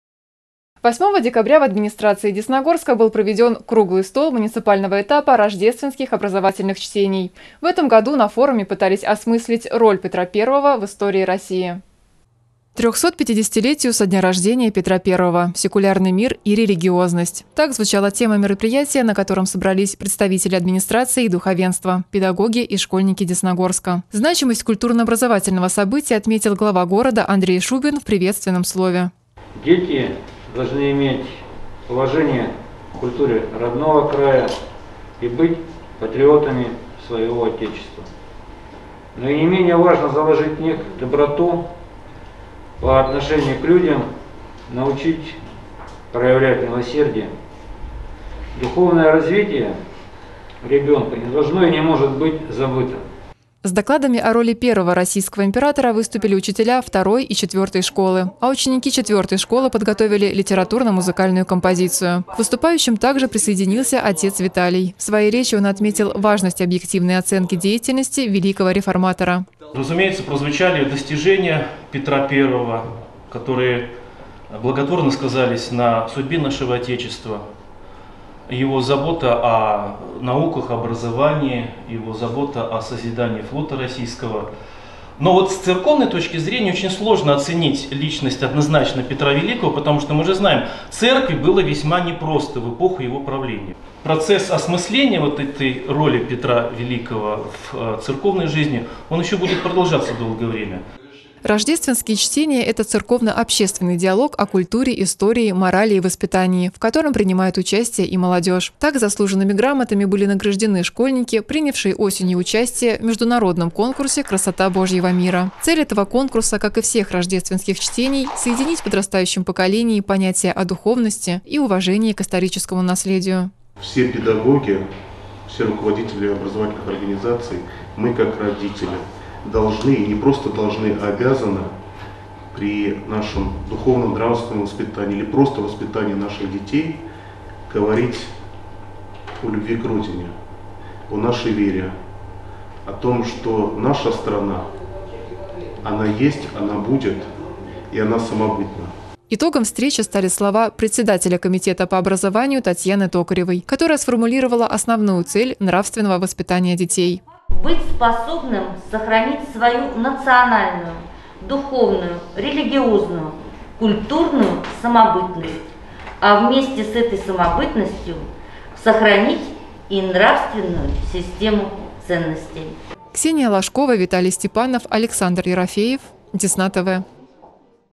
Десна-ТВ: В Десногорске состоялись региональные Рождественские чтения
8 декабря в Администрации Десногорска был проведен круглый стол муниципального этапа Рождественских образовательных чтений. В этом году на форуме пытались осмыслить роль Петра I в истории России.